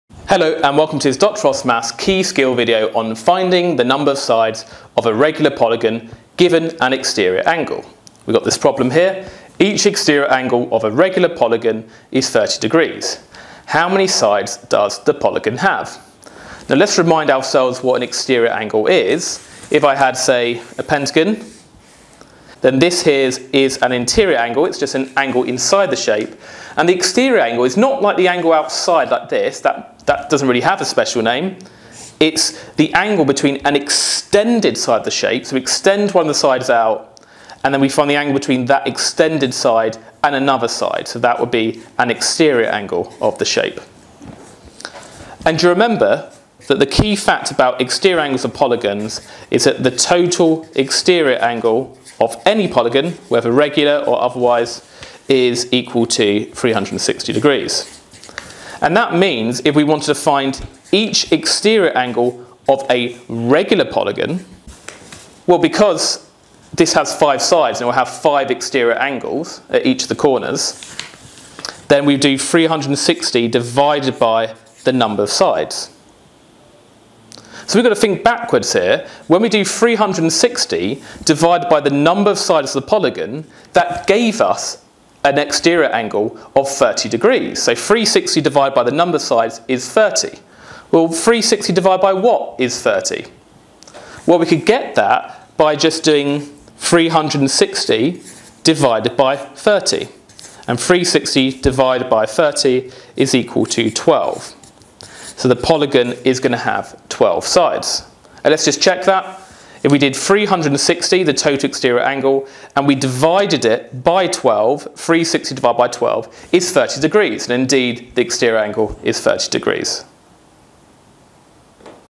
Key Skill - Find the number of sides of a regular polygon given the size of its exterior angles.
"Find the number of sides of a regular polygon given the size of its exterior angles."